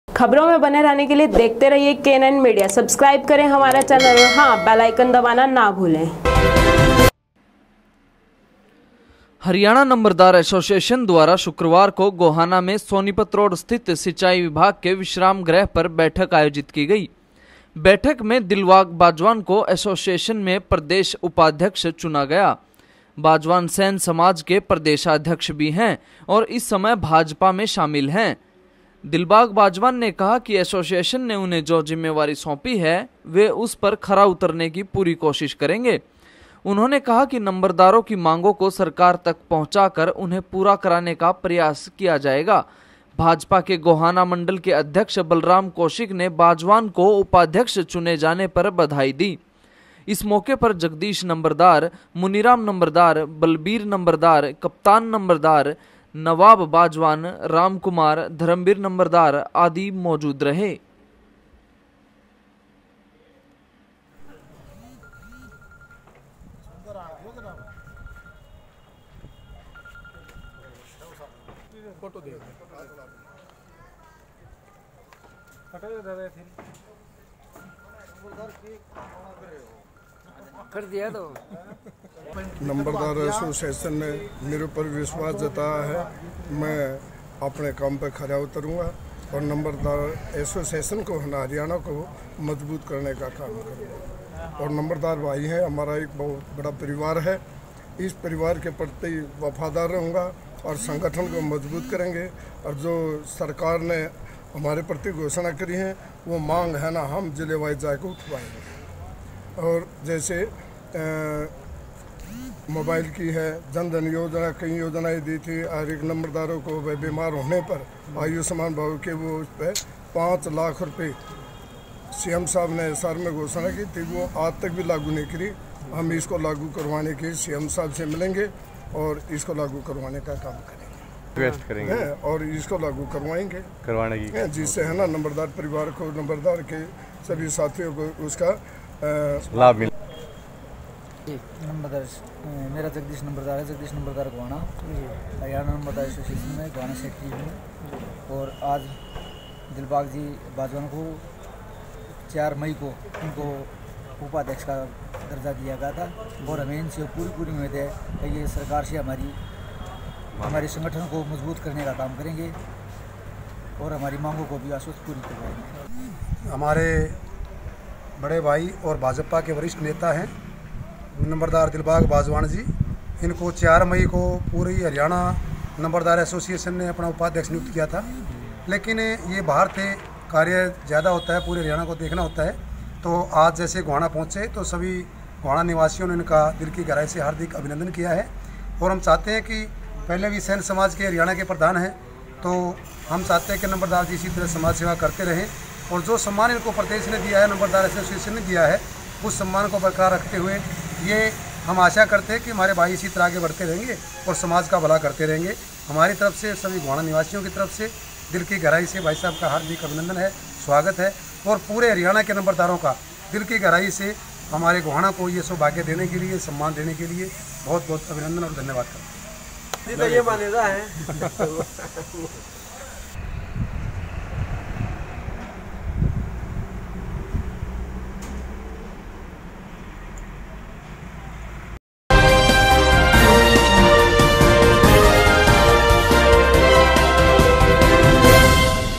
दिलबाग बाजवान बने नंबरदार एसोसिएशन के उपप्रधान
कहा जो जिम्मेवारी दी बखूबी निभाऊंगा

GOHANA II HARYANA NEWS